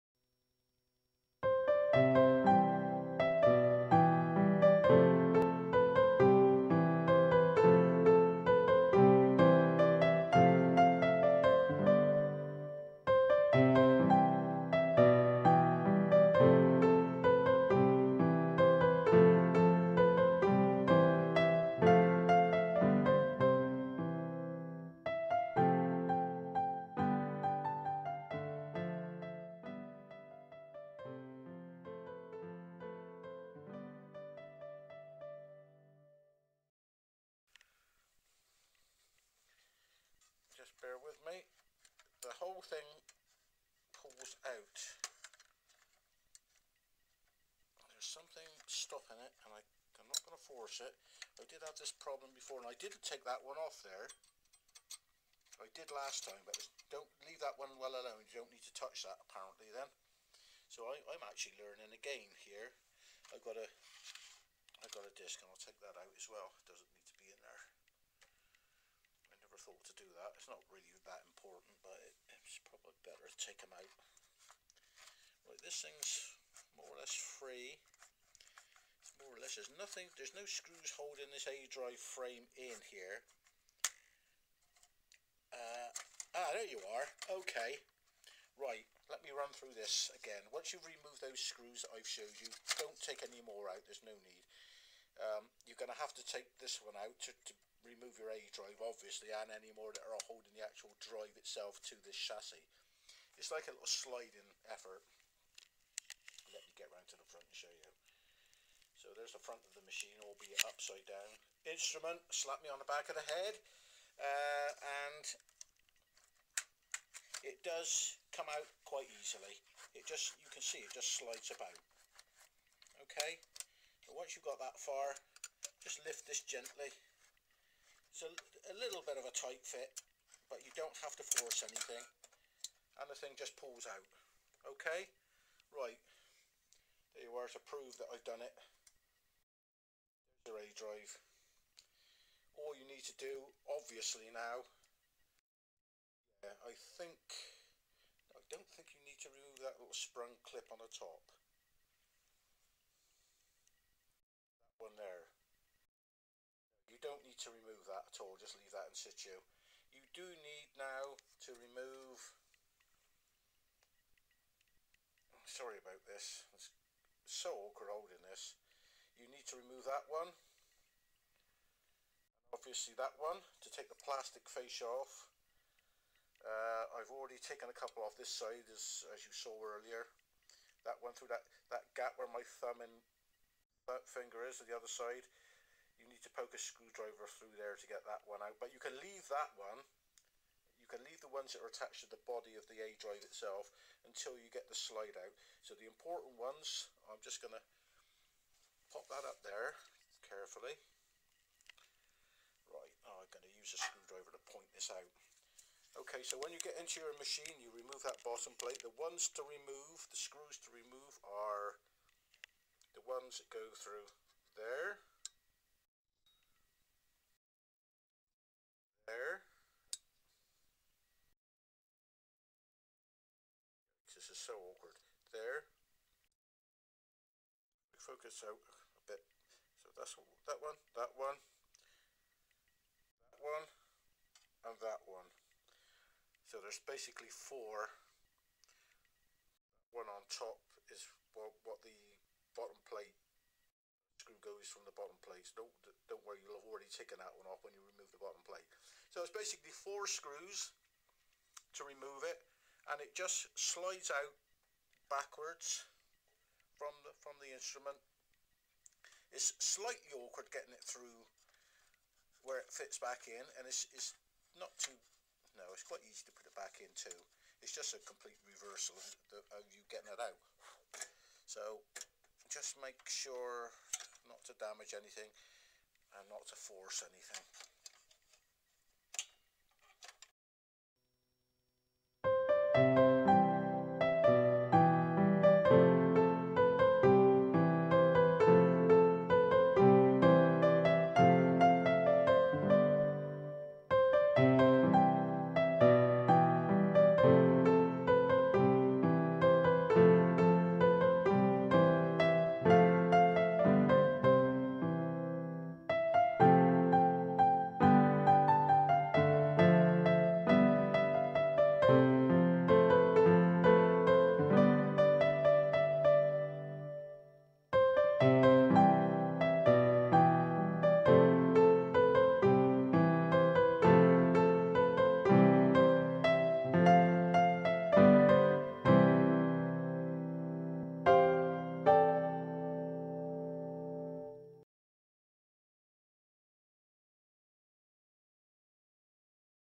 Trinity floppy change
A second, more detailed floppy/'A' drive change  'How To' video